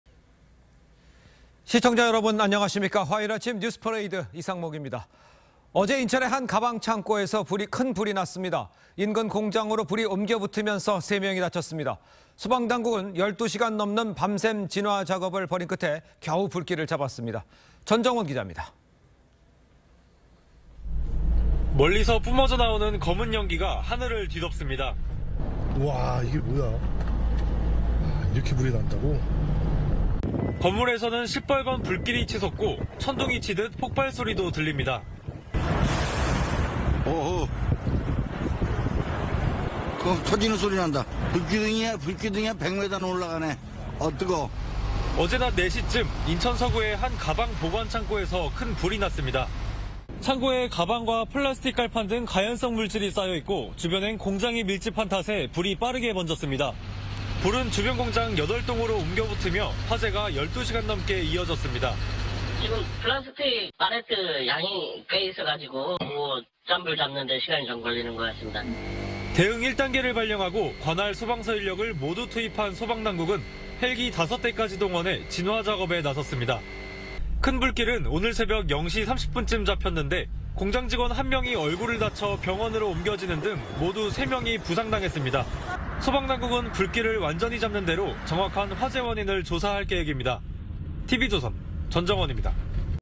인천 가방창고 화재 '3명 부상'…"12시간 이상 불 타"
어제 인천 의 한 가방창고 에서 큰 불 이 났습니다. 인근 공장으로 불이 옮겨 붙으면서 3명이 다쳤습니다. 소방당국은 12시간 넘는 밤샘 진화 작업을 벌인 끝에 겨우 불길을 잡았습니다.